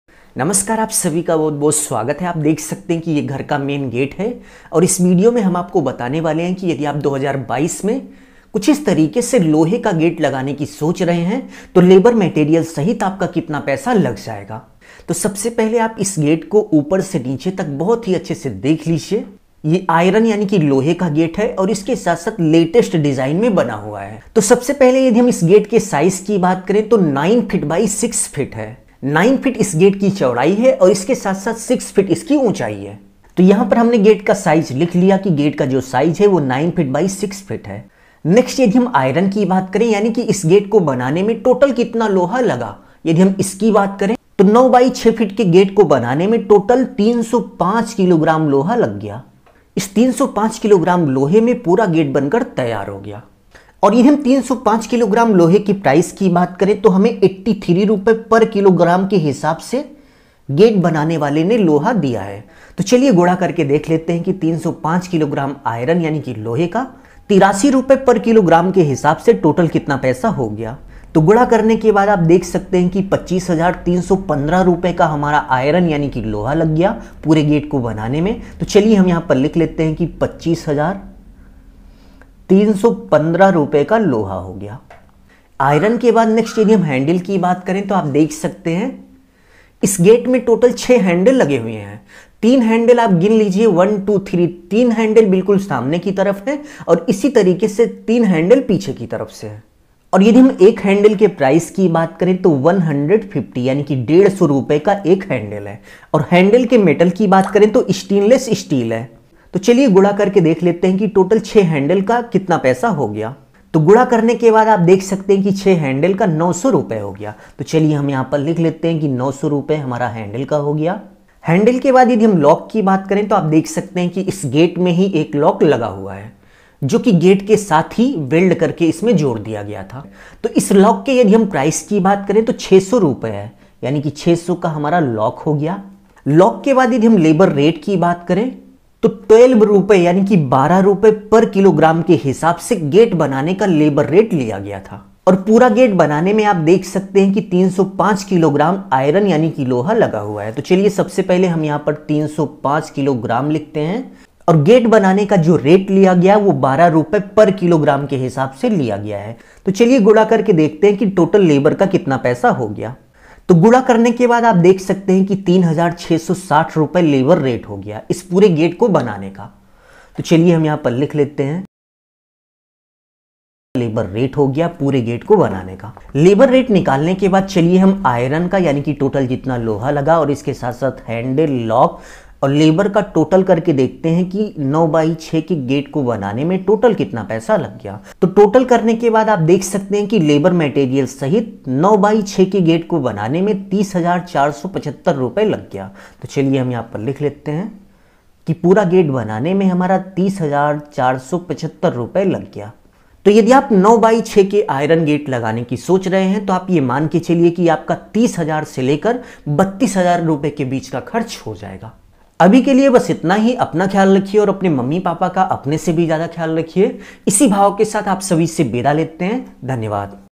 लोहे का मेन गेट लगाने में कितना खर्चा हो रहा है |iron gate price | gate design
वीडियो में क्या-क्या दिखाया गया है
गेट का डिजाइन
लोहे की गेट का साइज
गेट बनाने में कितना लोहा लगा
गेट के हैंडल का प्राइस
गेट में लॉक का प्राइस
लोहे का पूरा गेट बनाने में लेबर मटेरियल सहित कितना खर्च हुआ
Iron gate design for house
Gate installing price 2022, iron gate price per kilogram
Iron gate price 2022, men gate size,